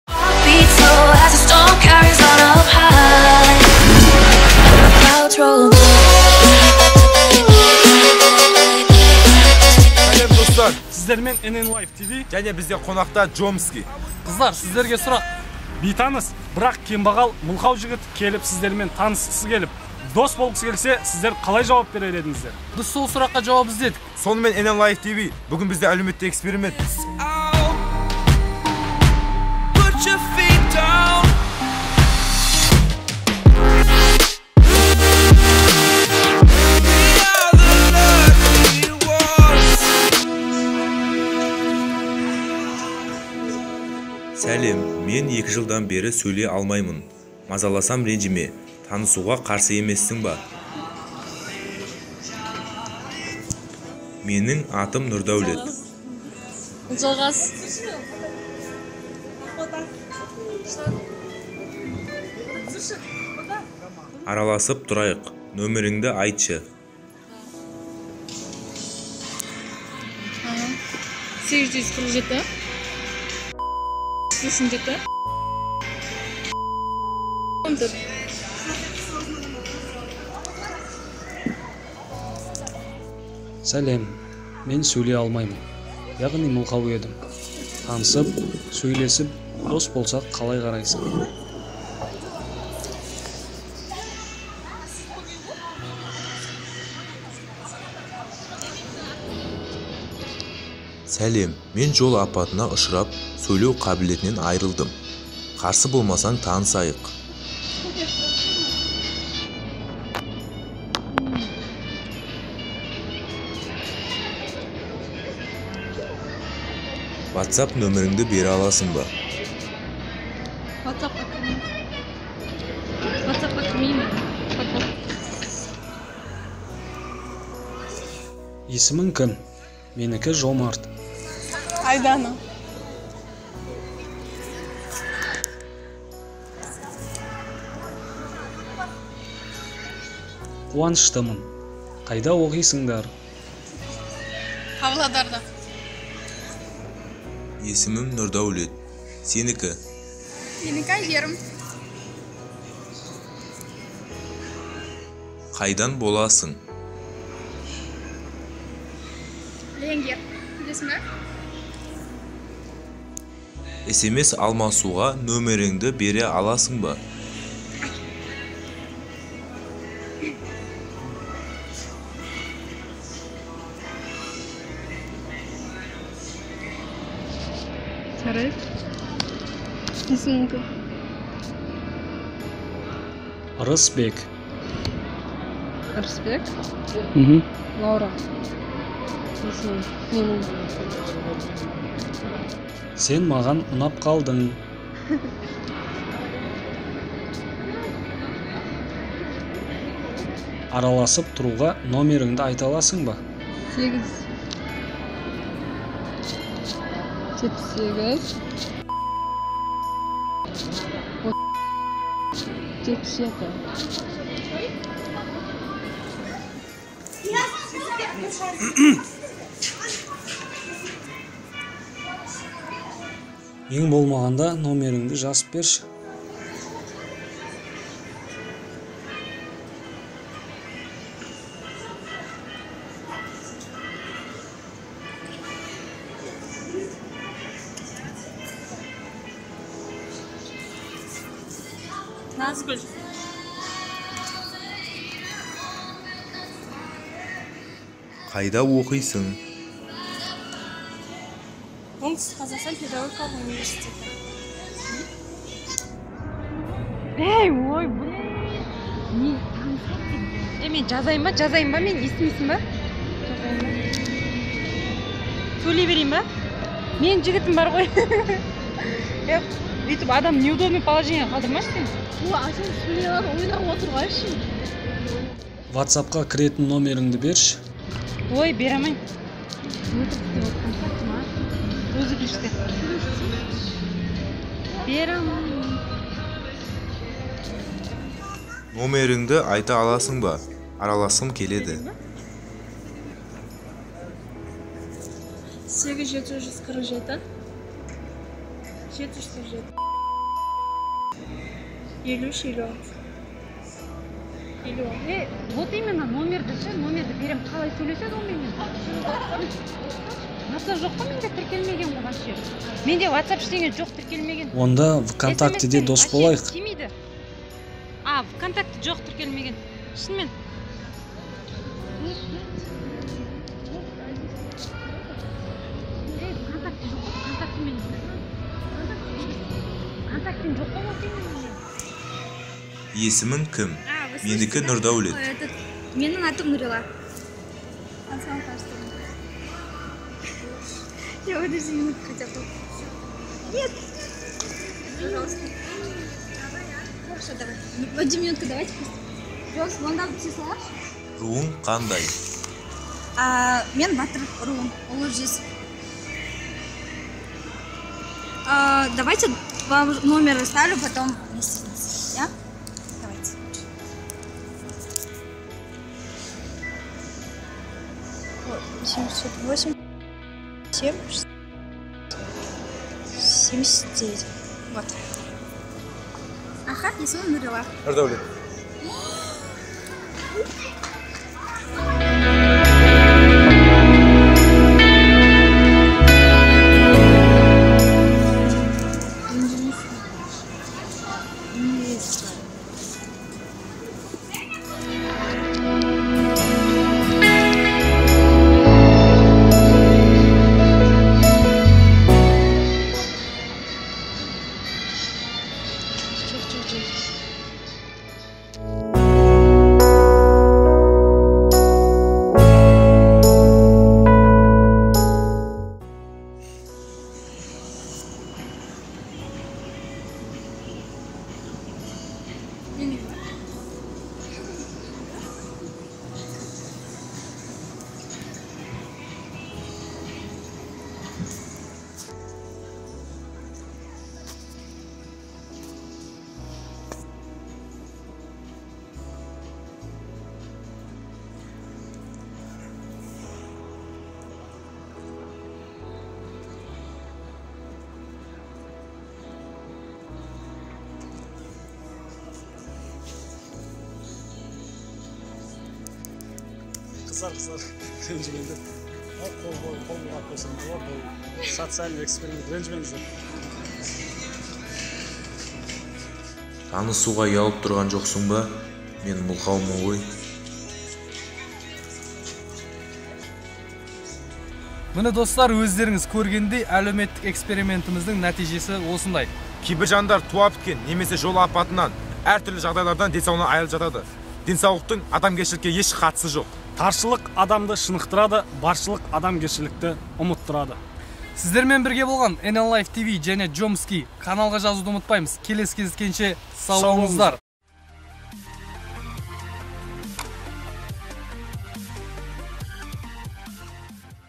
ҚАЗАҚСТАН МЫЛҚАУ ЖІГІТ Әлеуметтік эксперимент / НЕМОЙ ПРАНК КАЗАХСТАН / PRANK/NNNLIFETV
Сәлем Достар! Сізбен бейтаныс бірақ мылқау, кембағал адам келіп танысып, "Дос болғым келеді",-десе, қалай жауап берер едіңіздер? Біз сол сұраққа жауап іздеп көрдік!

Инстаграм парақшамыз: @nnnlifetv